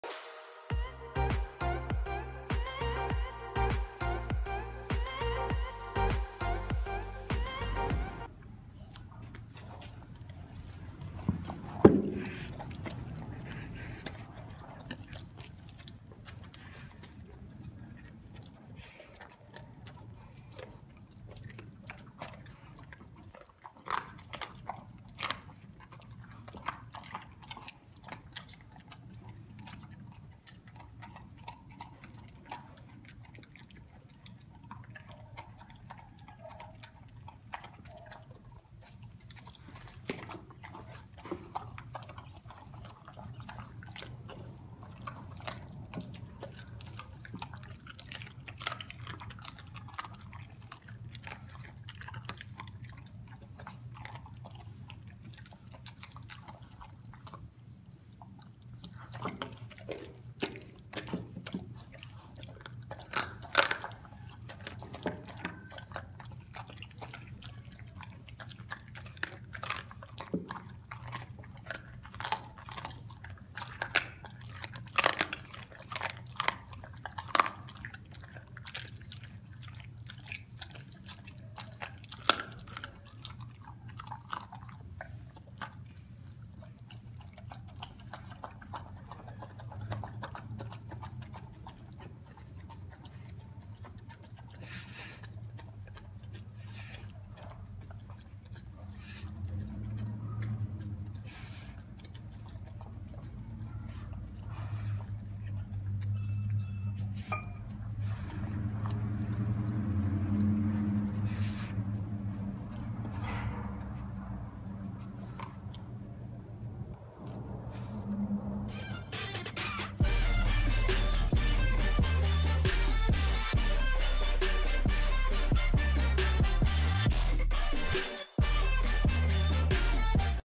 Raw feeding Doberman | Chicken - Ground Beef - Eggs
Kaia the raw fed Doberman eating raw chicken thigh, 2 eggs, and ground raw beef. Kaia is a 90lbs 2 year old female doberman eating 2-3 percent body weight split into two meals a day.

Interested in supporting our channel for free?! If you shop on amazon please use our